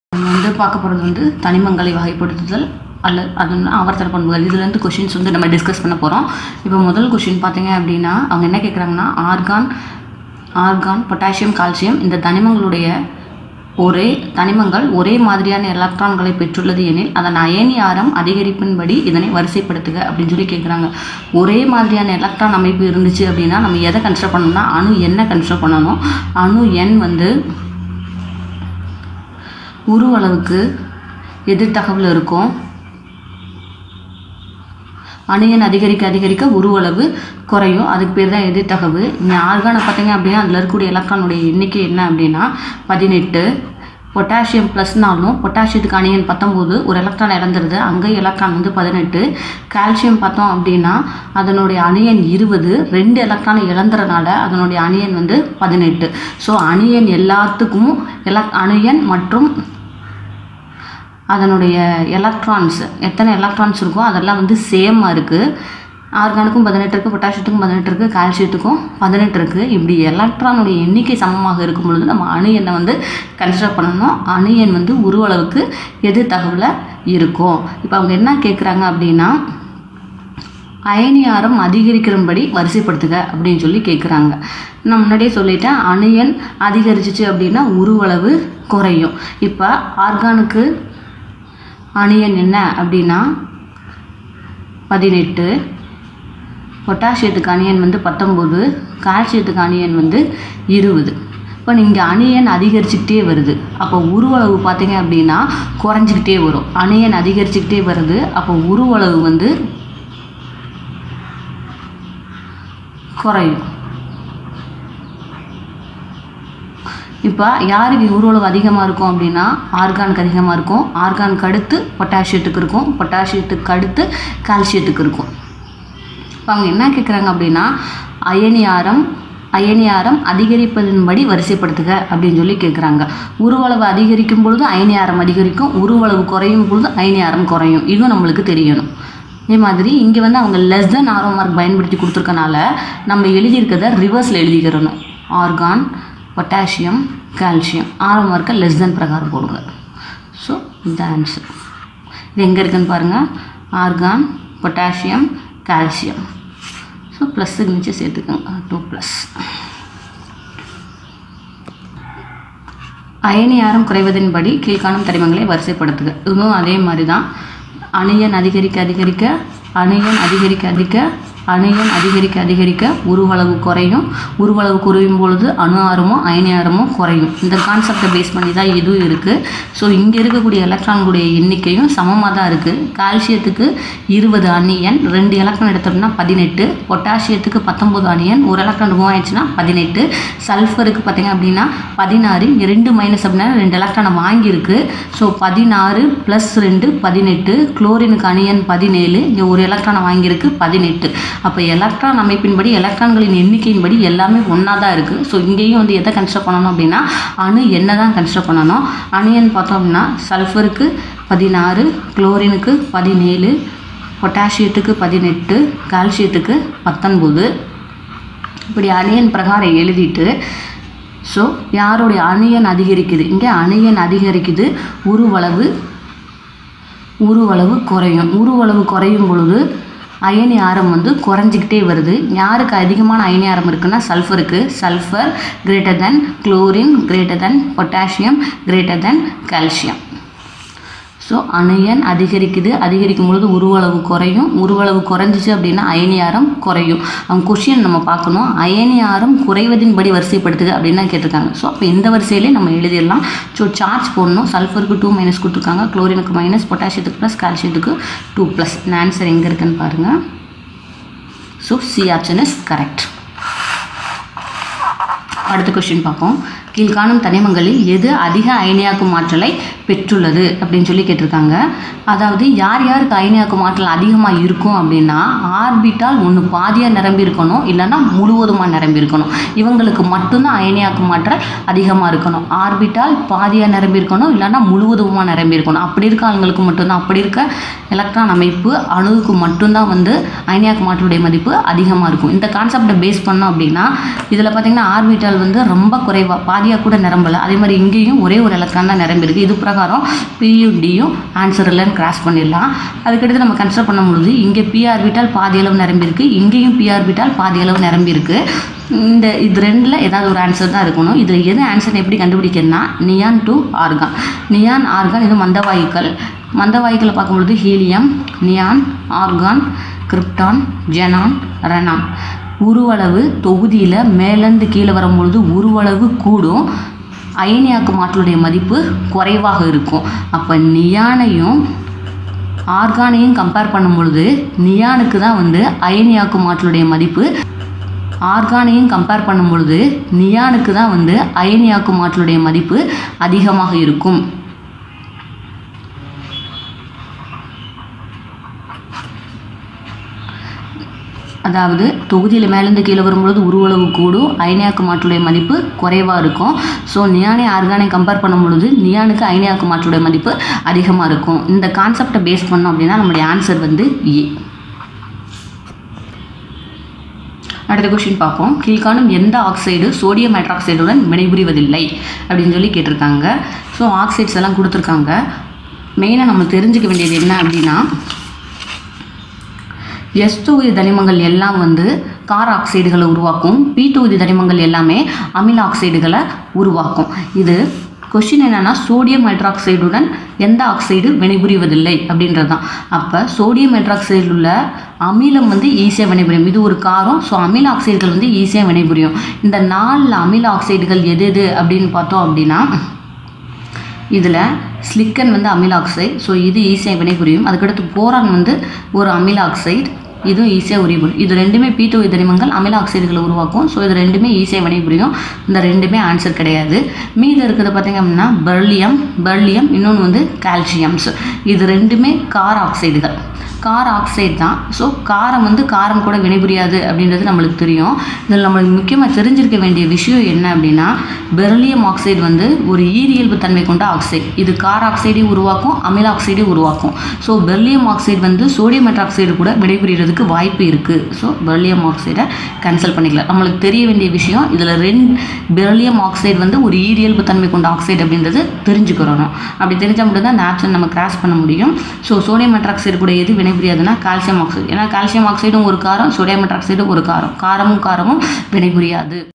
நீட் தேர்வு - தனிமங்களை வகைப்படுத்துதல் / ஆவர்த்தன பண்புகள் - Previous Year Questons Part 1
கடந்த 10 ஆண்டுகளில் வேதியியல் தனிமங்களை வகைப்படுத்துதல் / ஆவர்த்தன பண்புகள் பிரிவில் இருந்து கேட்கப்பட்ட கேள்விகளும் அதற்கான விளக்கமும் இந்த வீடியோவில் உள்ளது